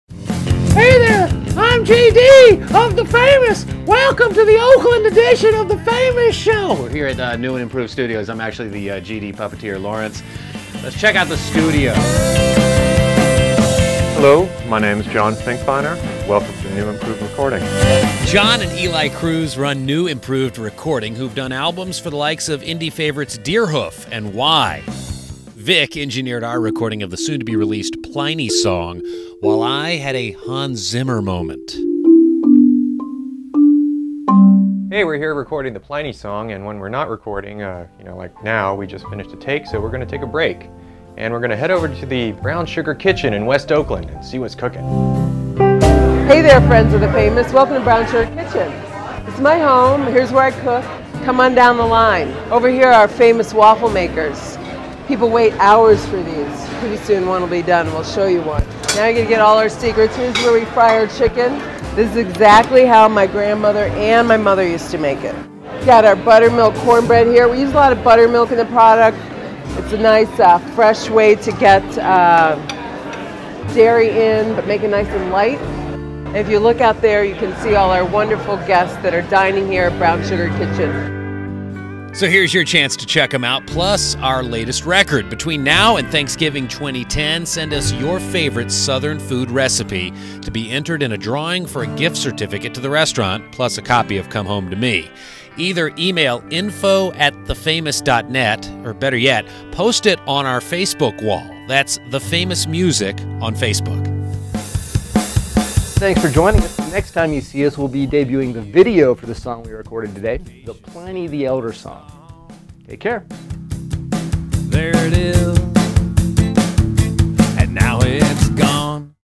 The Famous Show: Recipe Contest
The Famous take you around Oakland for a look inside the studios of New, Improved Recording during the 'Pliny the Elder' session... plus the Brown Sugar Kitchen with chef Tanya Holland for a special Southern Cooking Recipe Contest.